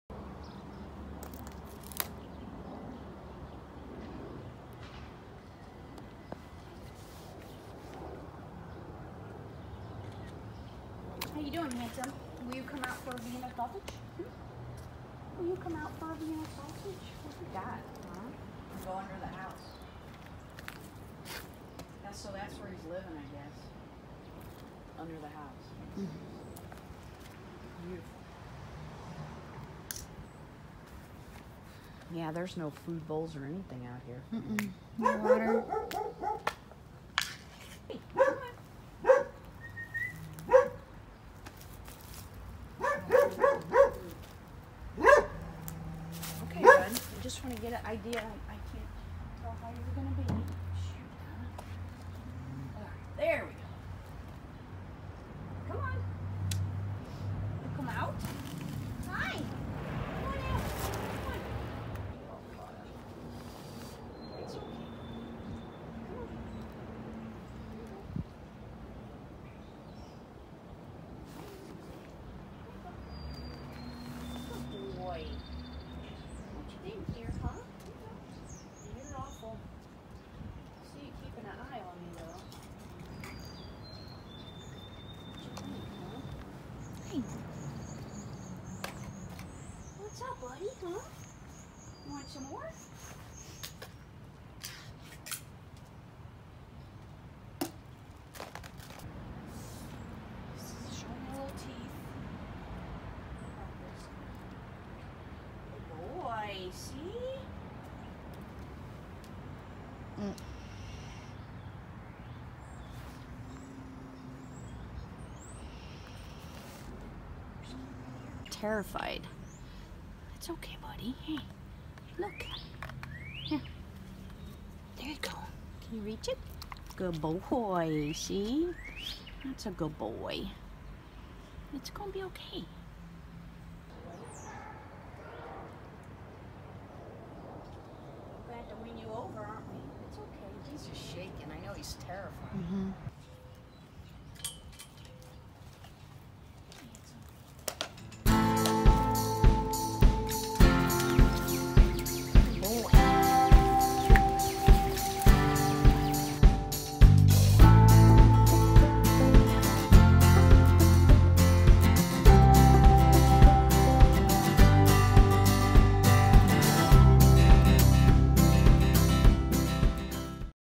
He's been alone for months - Stray Rescue of St.Louis
According to neighbors, he's been alone for months. "Are you going to save that dog?" they yelled to us. "I fed him back when it was really cold."
He shook so hard from fear he brought us to tears. He's very dehydrated and hungry. Meet Olson, now safe with us.